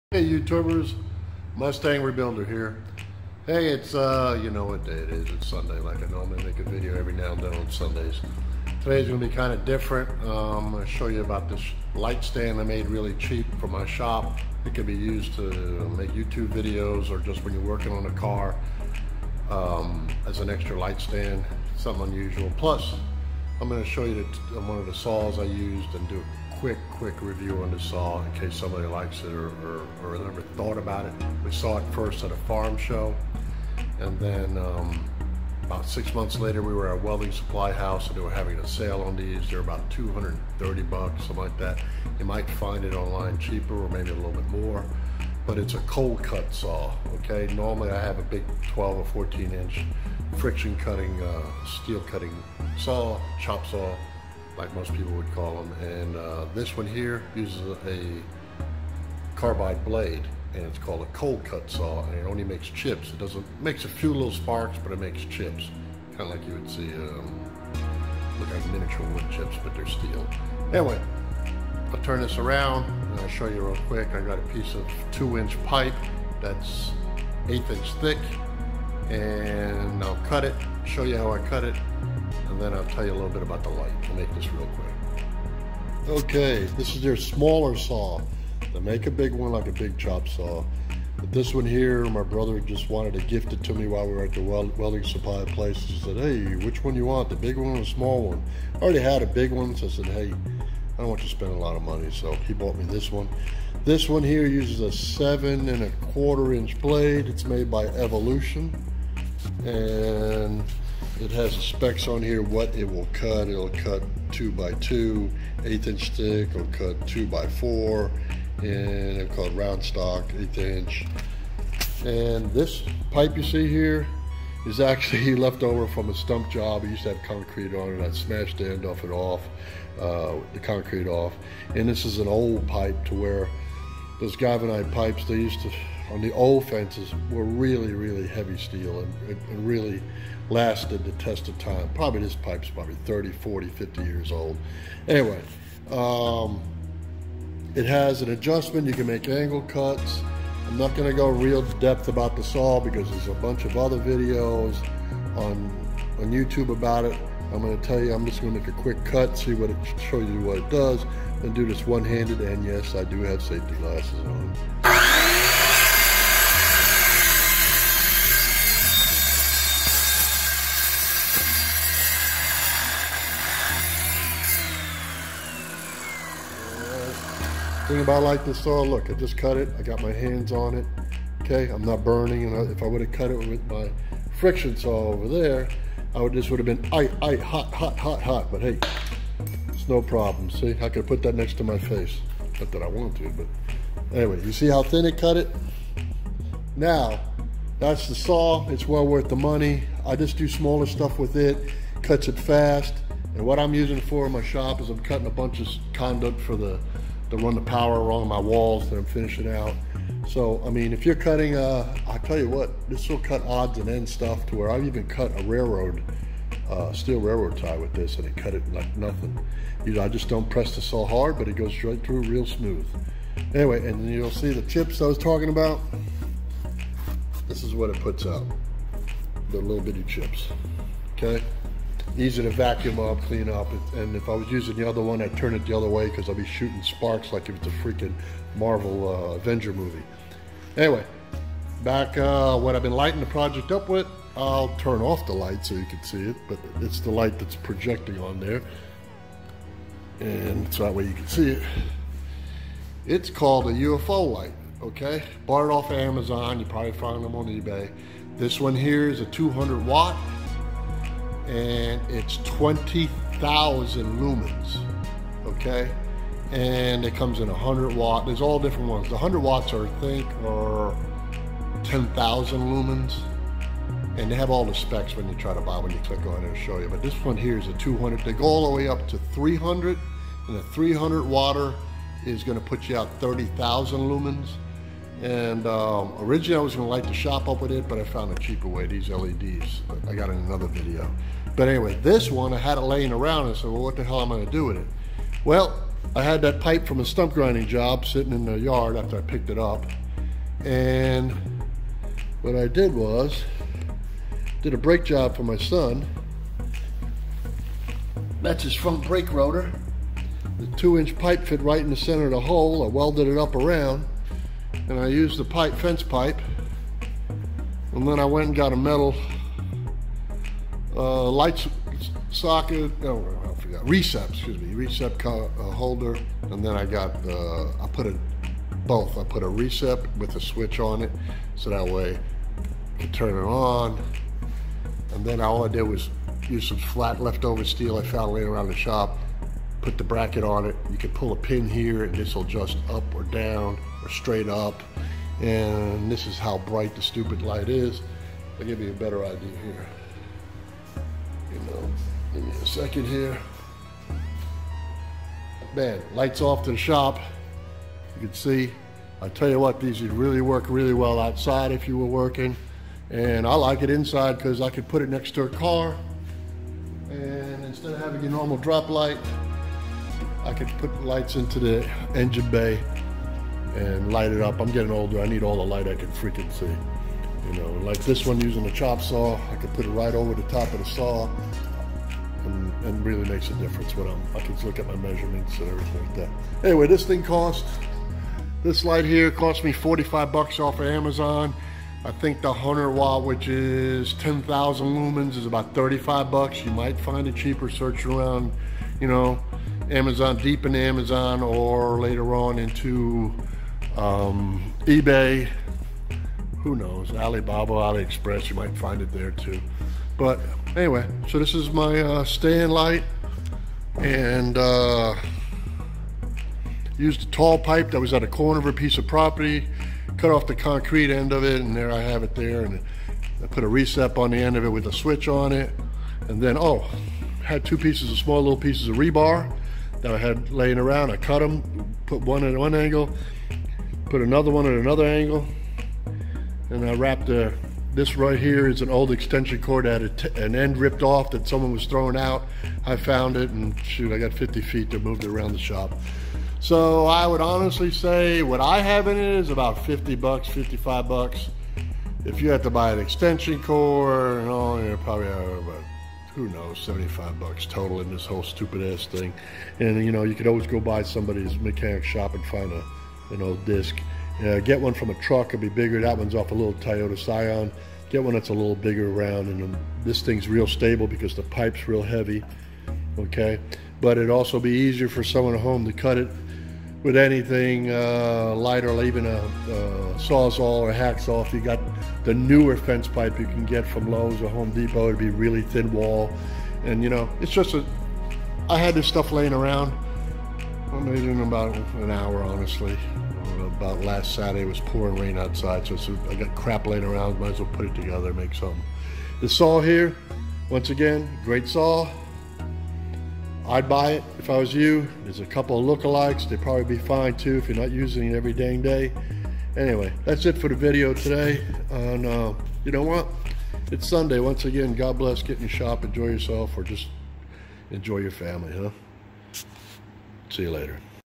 Evolution cold cut saw and LED DYI photo/shop light stand
Building a Led photo or shop light with a stand using the evolution cold cut saw and used scrap materials